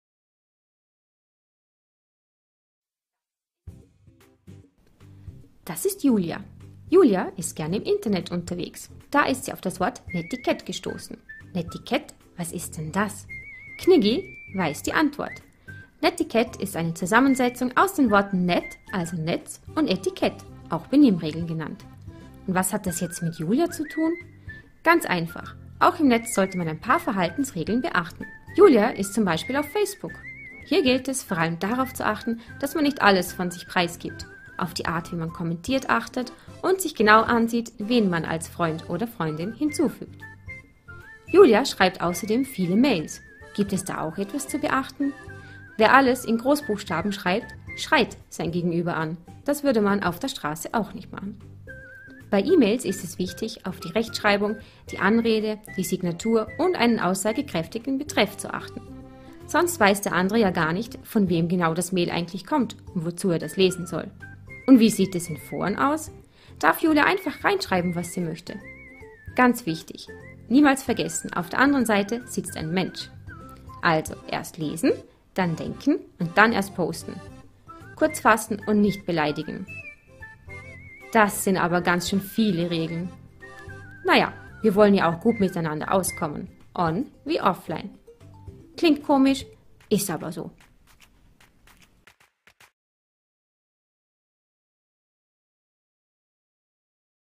Netiquette - einfach erklärt!
Diese Clip wurde im Rahmen eines REFLECT AND ACT! - Workshop von TeilnehmerInnen des Lehrgangs Freizeitpädagogik produziert.